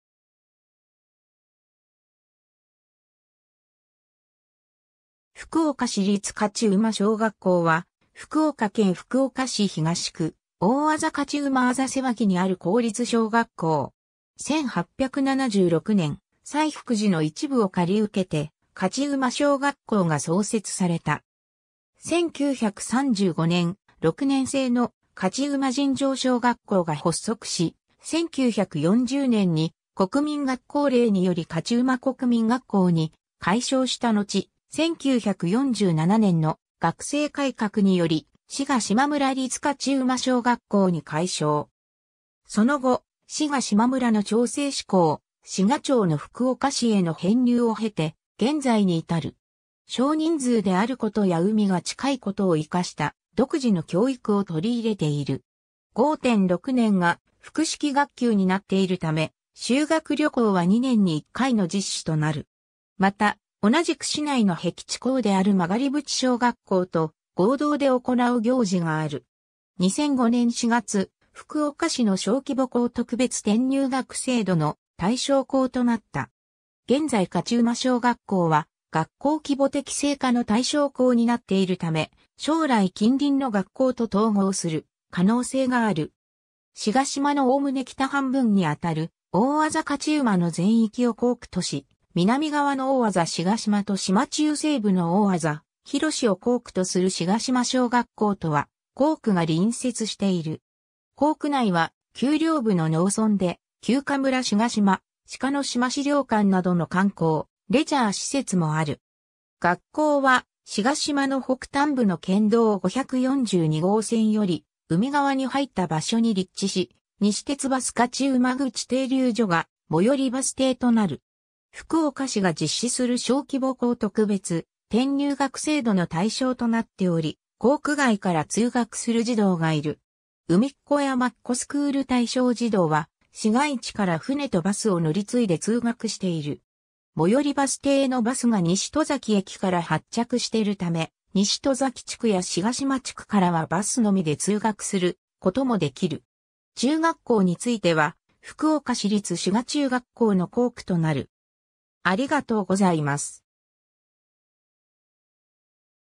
福岡市立勝馬小学校
福岡市立勝馬小学校（ふくおかしりつ かつましょうがっこう）は、福岡県福岡市東区大字勝馬字瀬脇にある公立小学校。
 1876年、西福寺の一部を借り受けて勝馬小学校が創設された。
1935年、6年制の勝馬尋常小学校が発足し、1940年に国民学校令により勝馬国民学校に改称したのち、1947年の学制改革により志賀島村立勝馬小学校に改称。
その後、志賀島村の町制施行、志賀町の福岡市への編入を経て現在に至る。
 少人数であることや海が近いことを活かした独自の教育を取り入れている。
 5・6年が複式学級になっているため、修学旅行は2年に一回の実施となる。
また、同じく市内のへき地校である曲渕小学校と合同で行う行事がある。
 2005年4月、福岡市の小規模校特別転入学制度 (通称：海っ子山っ子スクール) の対象校となった。
 現在勝馬小学校は学校規模適正化の対象校になっているため、将来近隣の学校と統合する可能性がある。
 志賀島の概ね北半分にあたる大字勝馬の全域を校区とし、南側の大字志賀島と島中西部の大字弘を校区とする志賀島小学校とは校区が隣接している。
校区内は丘陵部の農村で、休暇村志賀島・しかのしま資料館などの観光・レジャー施設もある。
 学校は志賀島の北端部の県道542号線より海側に入った場所に立地し、西鉄バス勝馬口停留所が最寄りバス停となる。
福岡市が実施する小規模校特別転入学制度 (通称：海っ子山っ子スクール) の対象となっており、校区外から通学する児童がいる。
海っ子山っ子スクール対象児童は市街地から船とバスを乗り継いで通学している。
最寄りバス停へのバスが西戸崎駅から発着してるため、西戸崎地区や志賀島地区からはバスのみで通学することもできる。
 中学校については、福岡市立志賀中学校の校区となる。